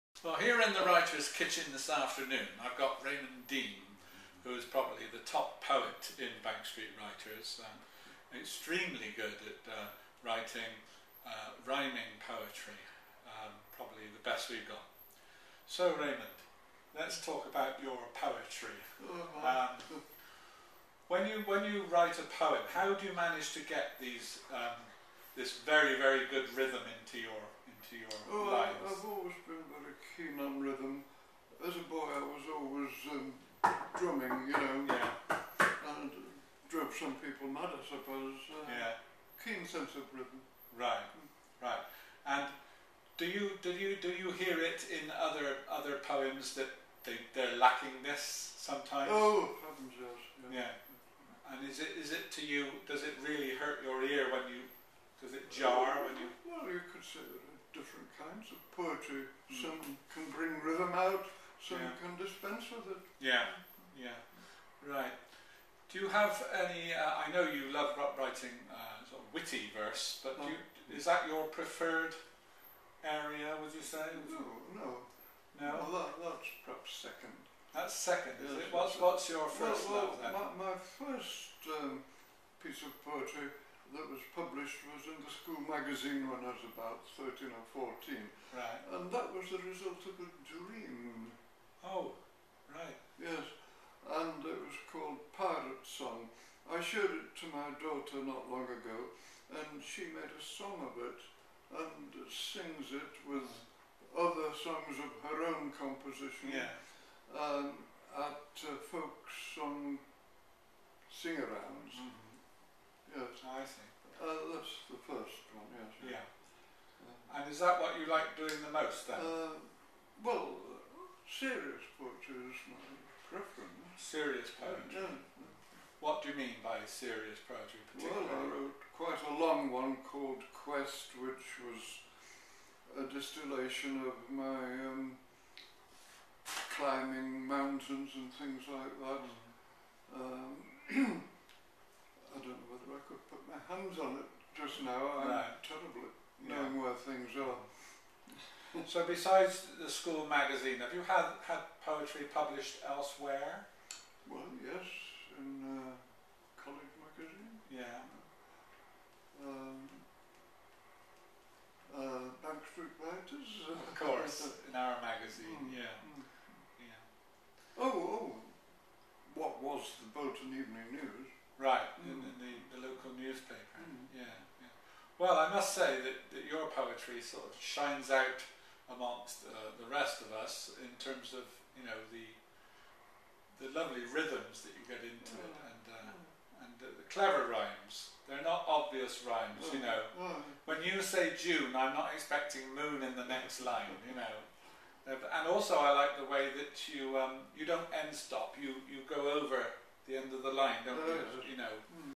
The Writer's Kitchen 14: Raymond Dean on poetry
James Hartnell talks to accomplished poet Raymond Dean about poetry and rhythm.